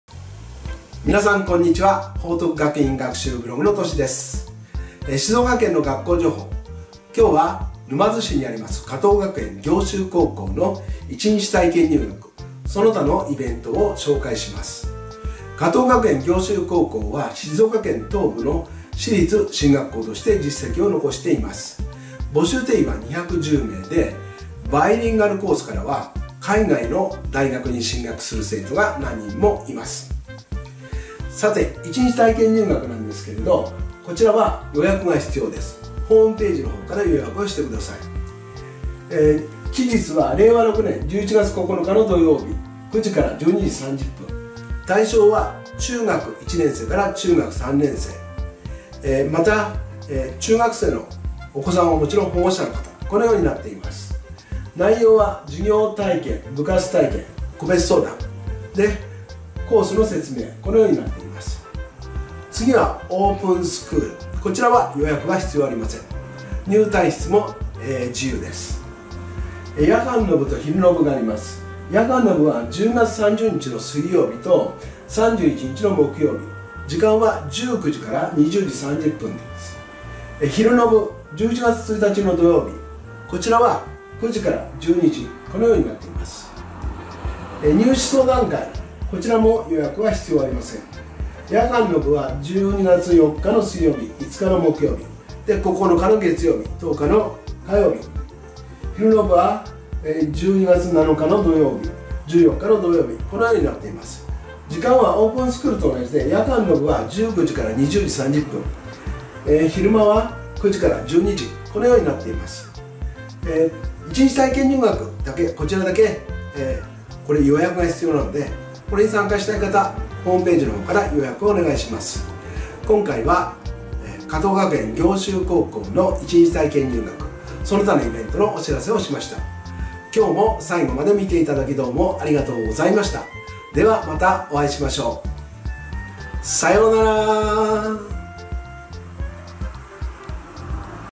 加藤学園暁秀高校1日体験入学・イベント紹介 静岡県の学校情報
加藤学園暁秀高校1日体験入学・イベントの紹介です。
加藤学園暁秀高校は、静岡県東部地区の私立進学校として実績を積み重ねている高校です。
特にバイリンガルコースからは、海外の大学に進学する生徒が何人もいます。